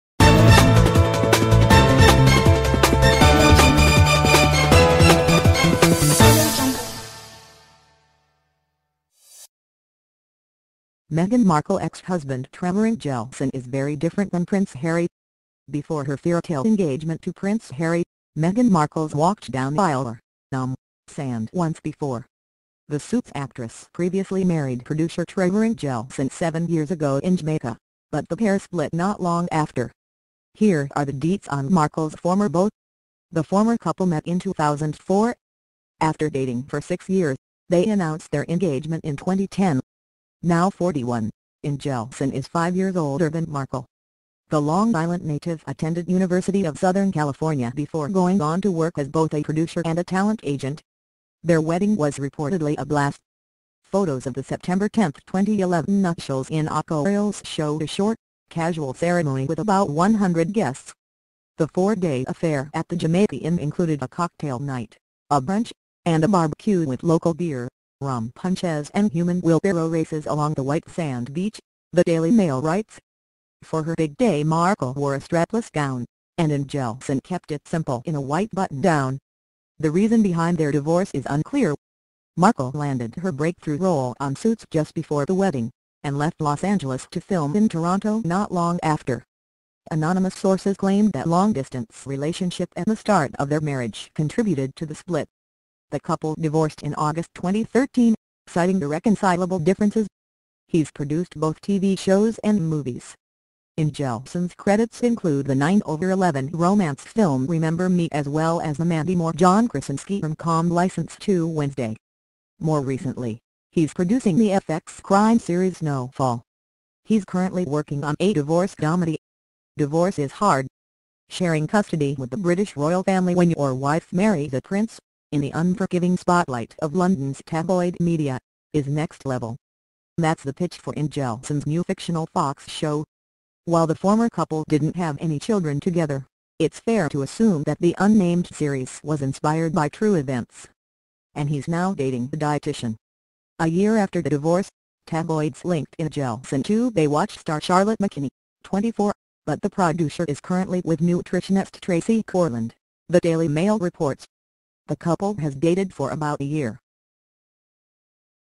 What is the difference between Meghan Markle's ex-husband and her husband?
Before her fairytale engagement to Prince Harry, Meghan Markle's walked down the aisle (or, um, sand) once before. The Suits actress previously married producer Trevor Engelson seven years ago in Jamaica, but the pair split not long after.